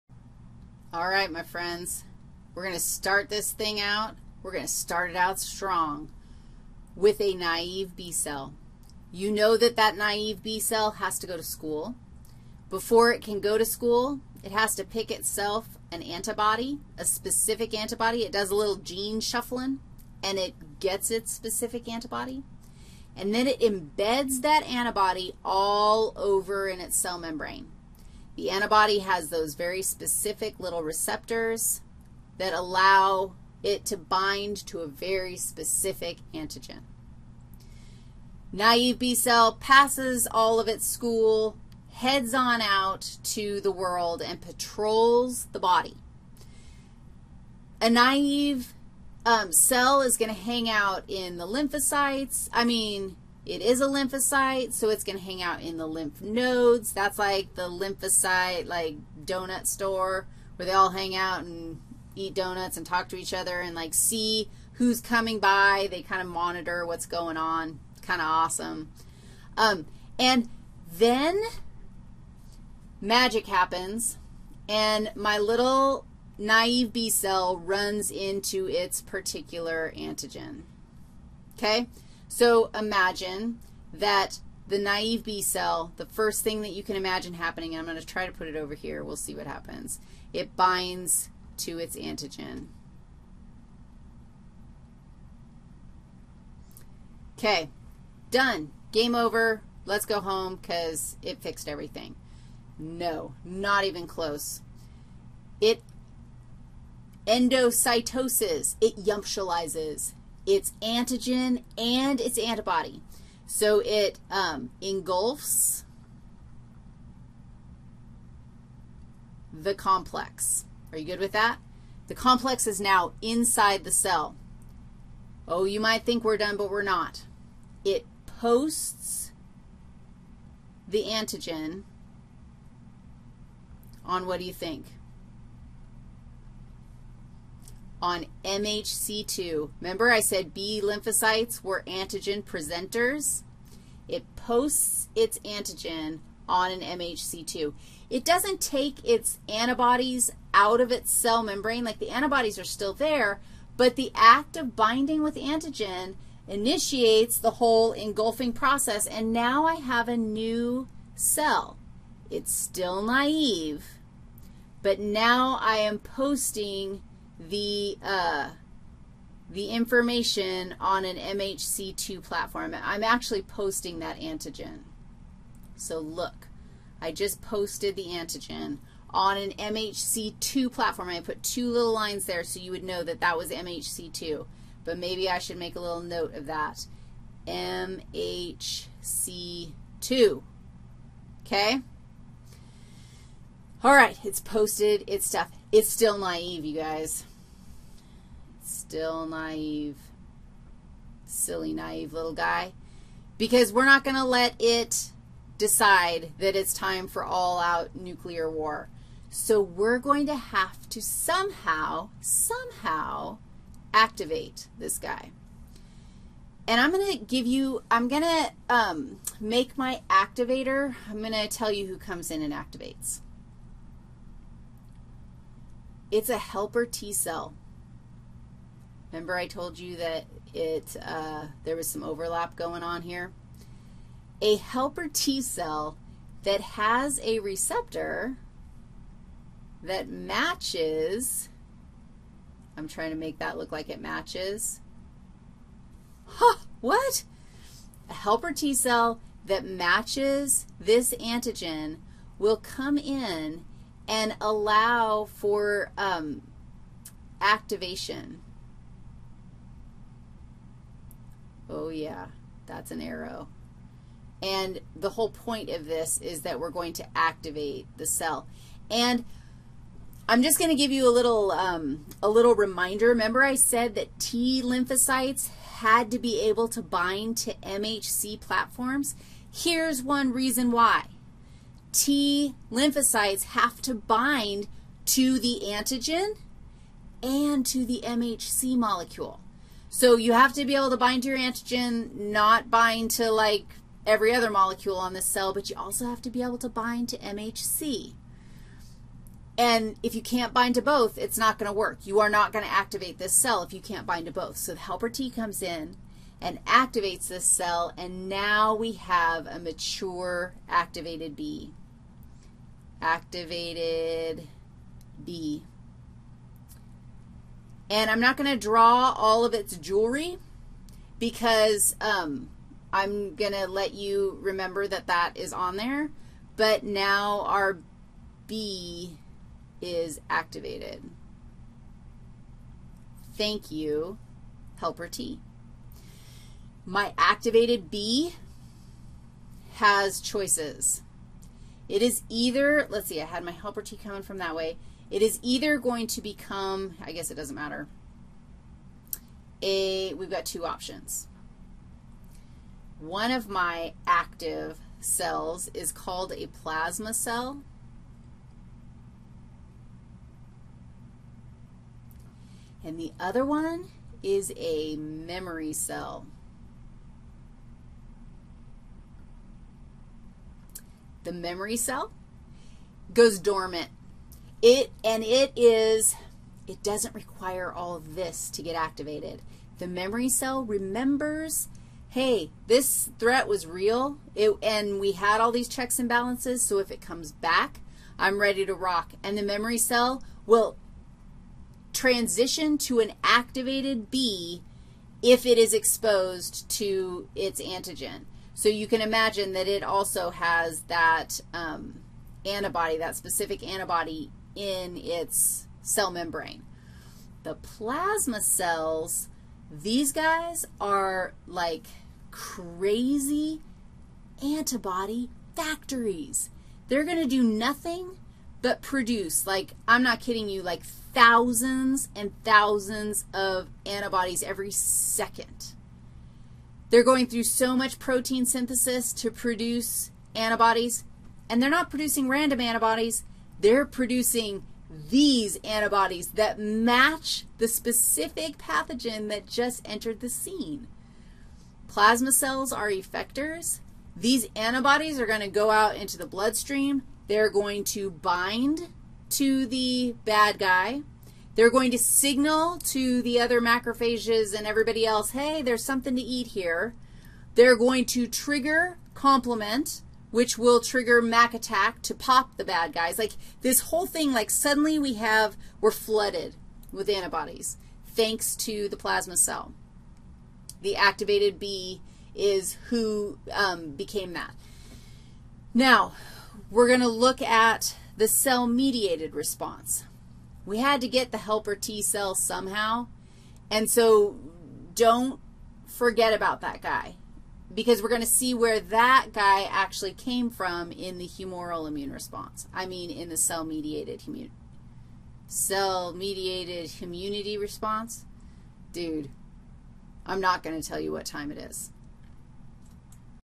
Acquired Immunity 5- Humoral
Part 4 in a 8 part lecture on ACQUIRED IMMUNITY in a flipped Human Physiology course taught by Wendy Riggs. CC-BY. Watch the whole lecture (all 8 videos) by going to the PLAYLIST